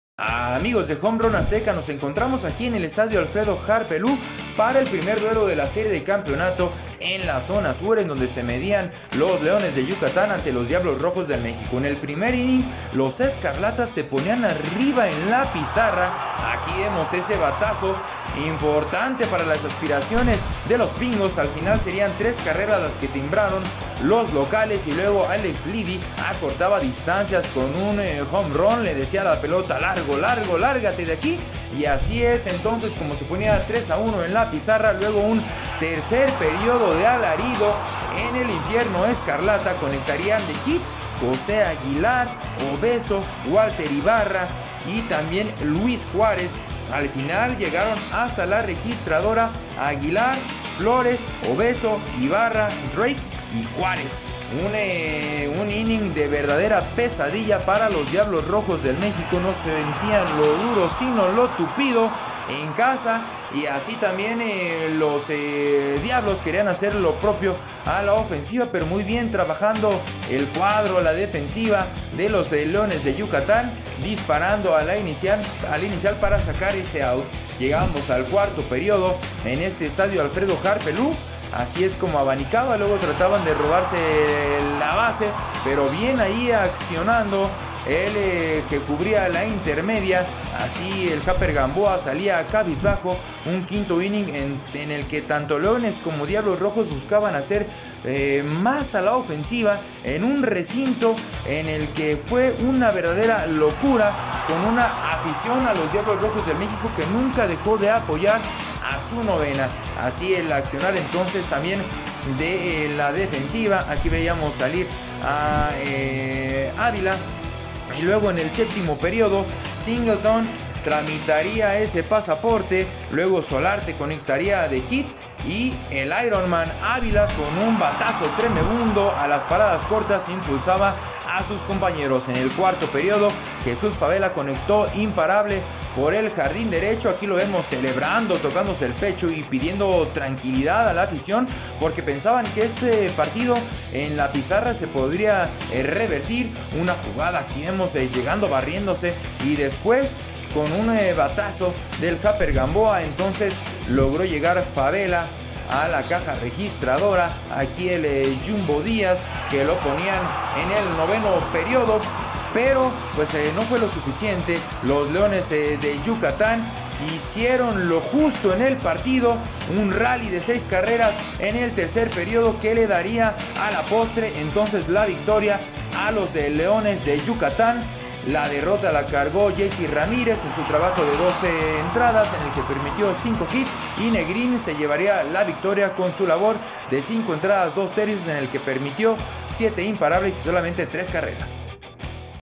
Leones de Yucatán 7-6 Diablos Rojos del México | Resumen completo | Liga Mexicana de Beisbol
Disfruta las mejores jugadas del partidazo entre Leones de Yucatán y los Diablos Rojos del México, en donde los felinos se llevaron el triunfo en el primer partido de la Serie de Campeonato de la Zona Sur.